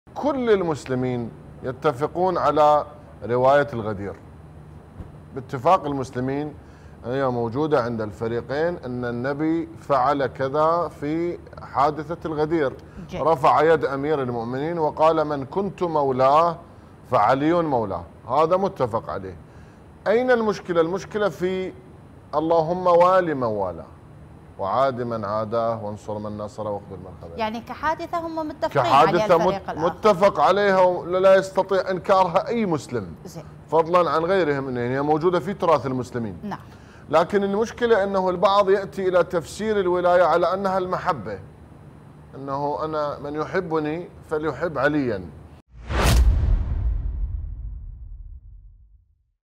مشكلتهم "والي من والاه".. الشيخ نور الساعدي: لا يستطيع أي مسلم إنكار رواية الغدير
الشيخ نور الساعدي/ باحث اسلامي